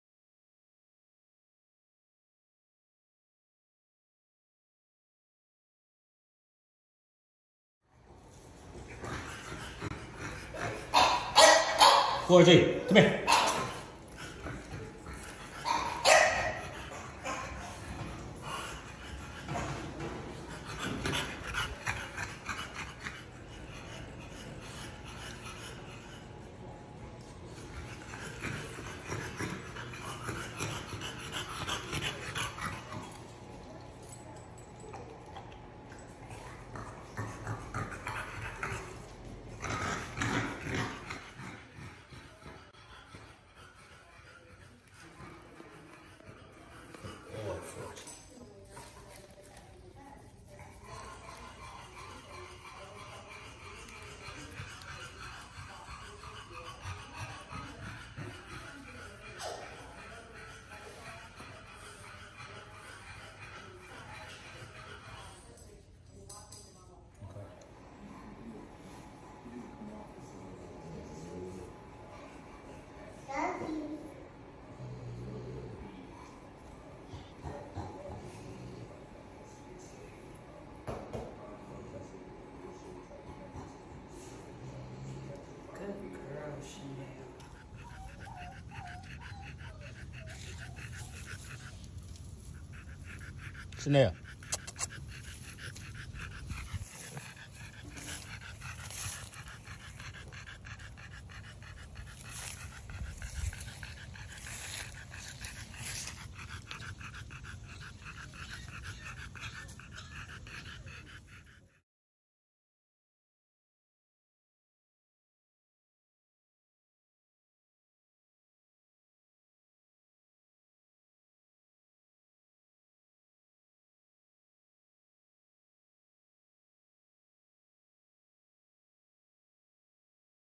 French Bulldog Artificial Insemination/Natural Breeding Forgi X Chanel
Breeding Forgi the chocolate Merle X Chanel the blue fawn pups due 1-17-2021 follow us @reedsfrenchieconnect on IG and Facebook